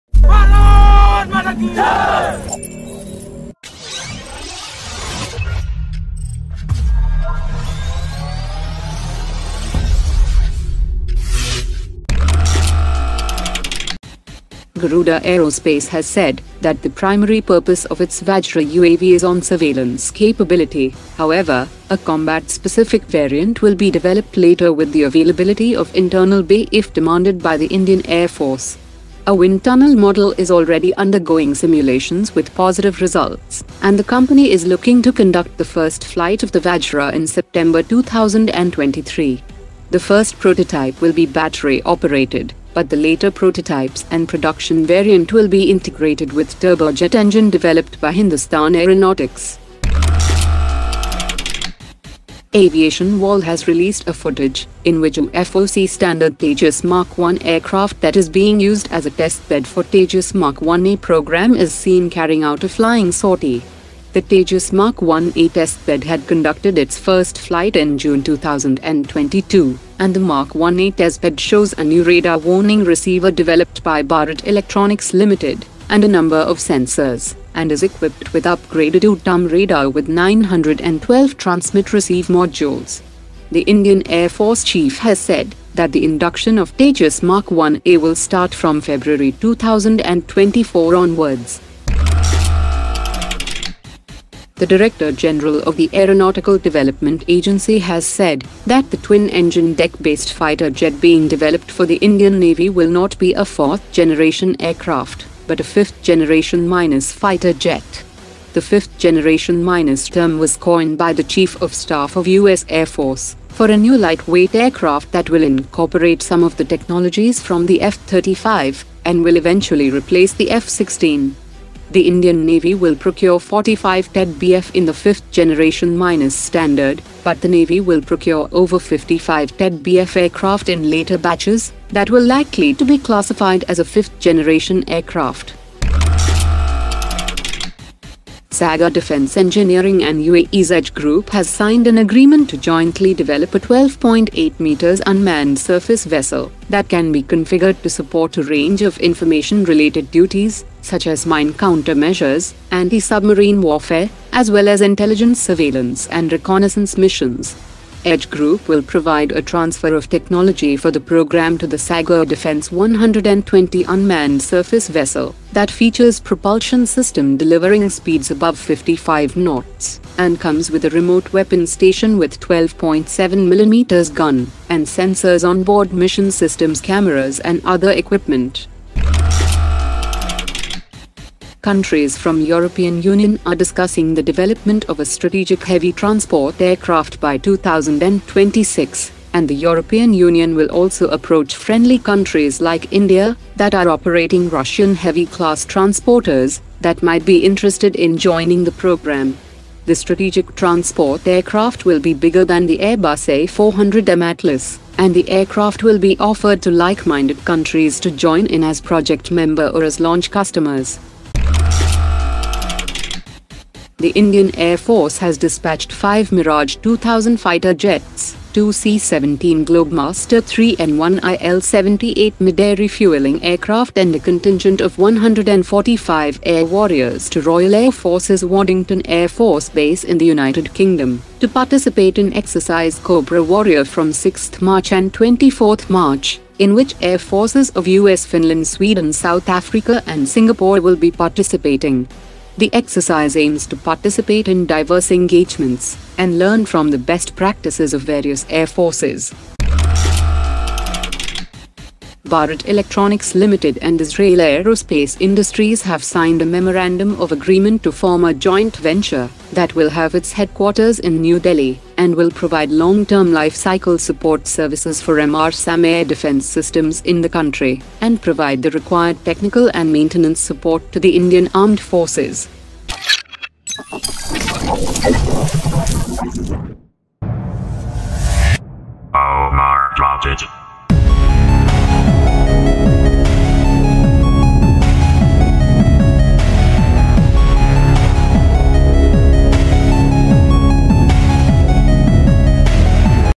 Indian Defence Updates : Tejas MK1A 1st Flight,Vajra UAV Ready,5th Gen TEDBF,IAF New MTA Offer
1. Garuda Aerospace's VajraUAV to conduct first flight in september 2023.
2. TejasMark1A testbed 1st flight
3. TEDBF to be classified as 5th gen minus : ADA Chief
4. Sagar Defence & Edge to work on unmanned surface vessel.
5. EU to approach India to join heavy transporter program.
6. IAF to participate in multilateral air exercise Cobra Warrior
7. BEL & Israel Aerospace Industries to form joint venture for MRSAM support to India's armed forces.